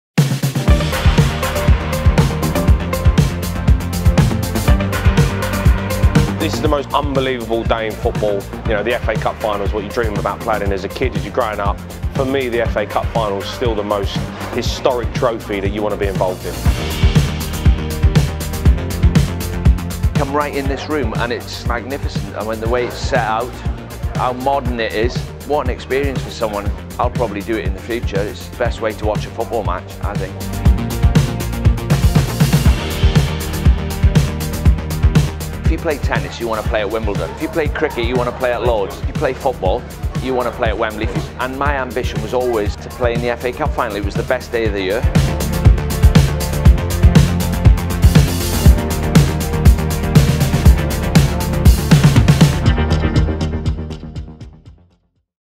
FA Cup Final 2022 | Inside The Green Room at Wembley
The Green Room made its debut at Wembley Stadium, and what a way to do it with the FA Cup Final 2022 as Chelsea took on Liverpool! Joining us alongside Green Room host Mark Durden-Smith was none other than football legends Jamie O'Hara and Dean Saunders.

Our exclusive hospitality suite gave guests a pitch view with seats directly outside the restaurant to enjoy every second of the game.